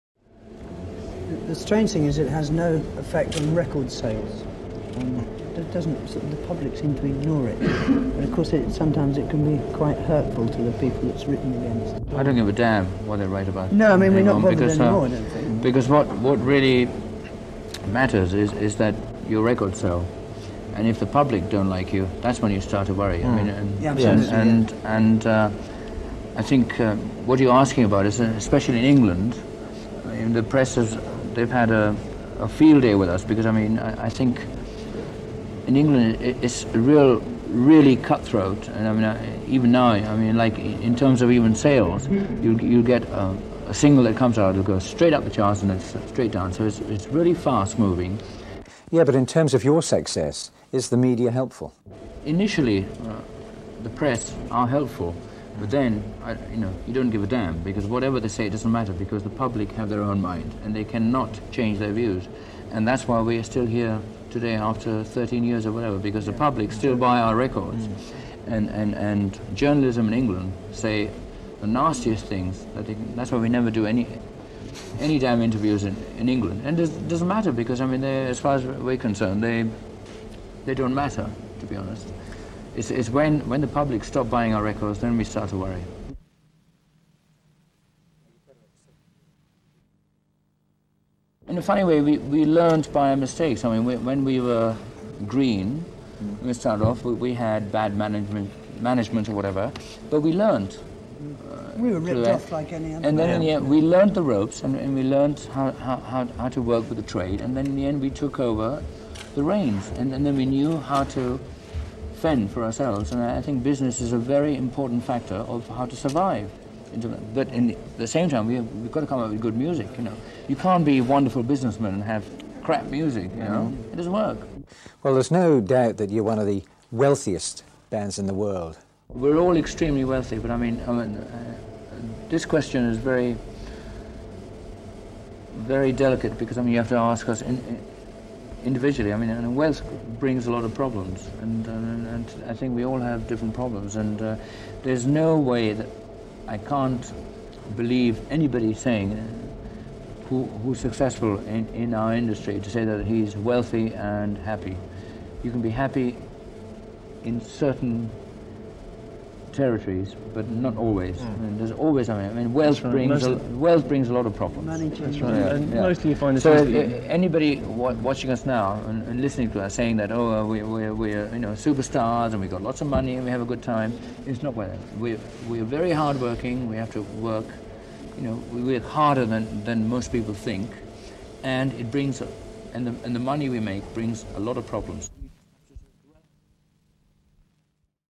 Queen - 1984 Dylan Taite NZ interview (RARE)
Courtesy of the Taite Family archives, NZ music journalist interviewed Queen backstage ahead of their Auckland concert in 1984, with Freddie Mercury in excellent form describing life in one of the world's greatest bands ever.
*reproduced with permission from the Taite Estate